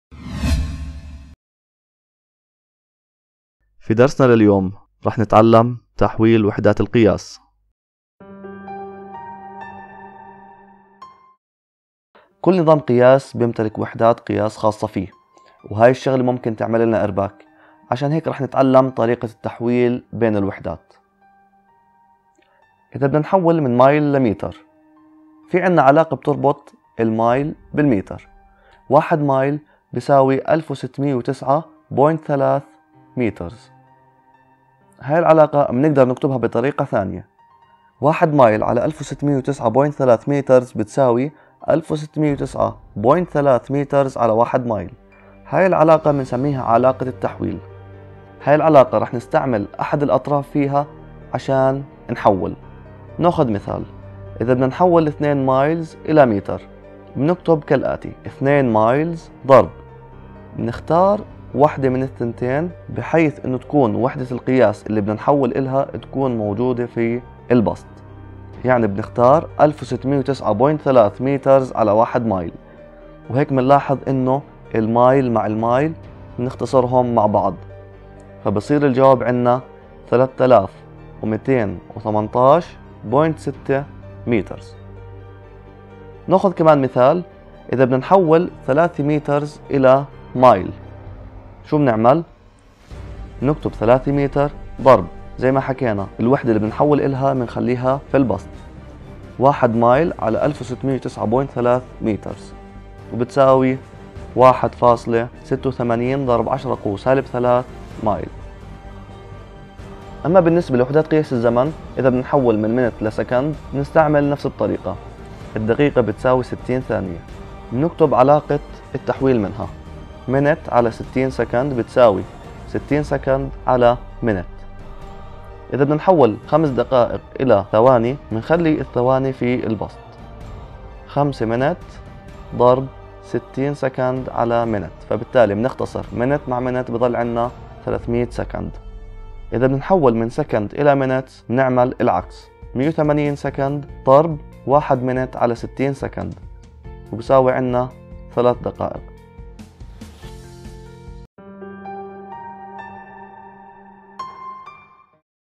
فيزياء عامة 101 تحويل الوحدات – CH1 Physics 101 Unit Conversion
فيزياء 101
إعداد و تقديم :: أحمد صبحي

تشابتر 1 :: الفيزياء و القياس
تحويل الوحدات
من كتاب سيروي

إنتاج الفيزياء للجميع A+ FYsics

Physics 101
Presented By :: Ahmed Subhi

Physics for Scientists and Engineers by Raymond A. Serway and John W. Jewett
Chapter 1 :: Physics and Measurement
Unit Conversion